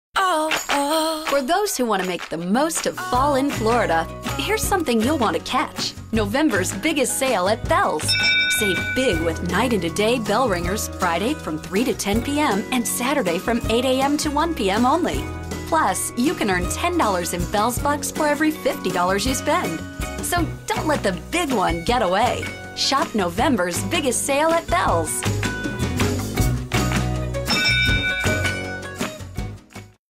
Alex Madera in Bealls Commercial
Alex can be seen fishing at the end of this Bealls commercial.